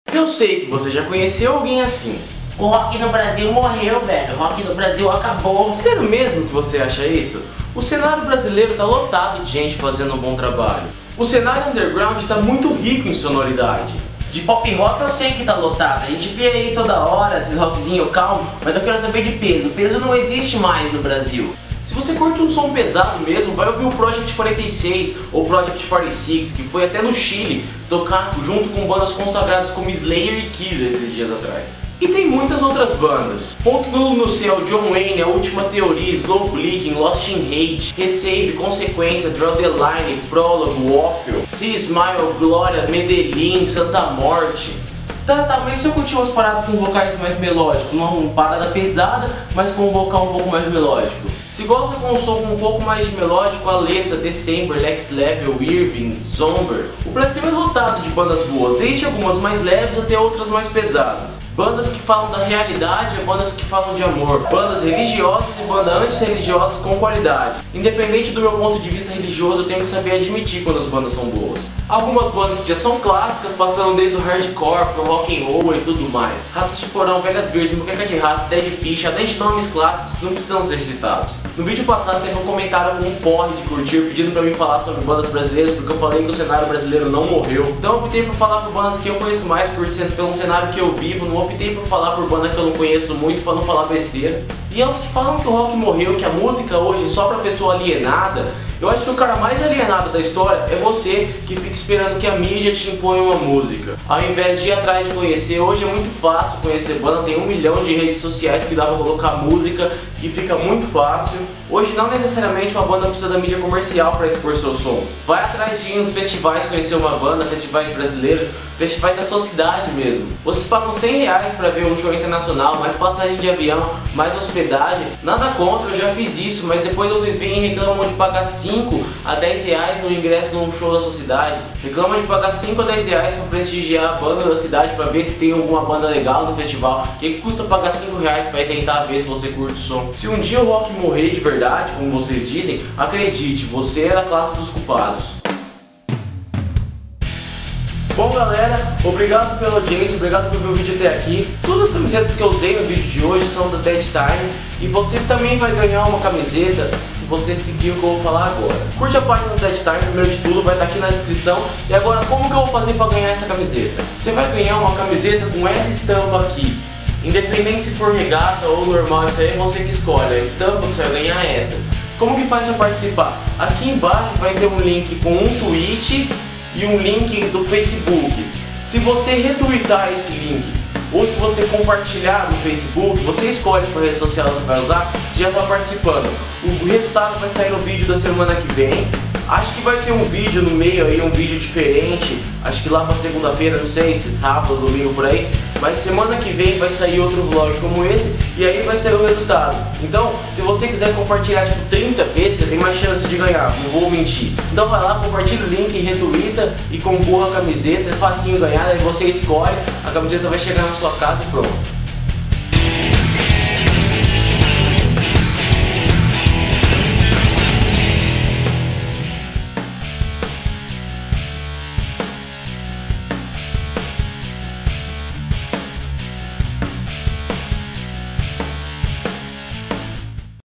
O ROCK BRASILEIRO NÃO MORREU!
Um vídeo direcionado pra quem diz que o cenário brasileiro está morto.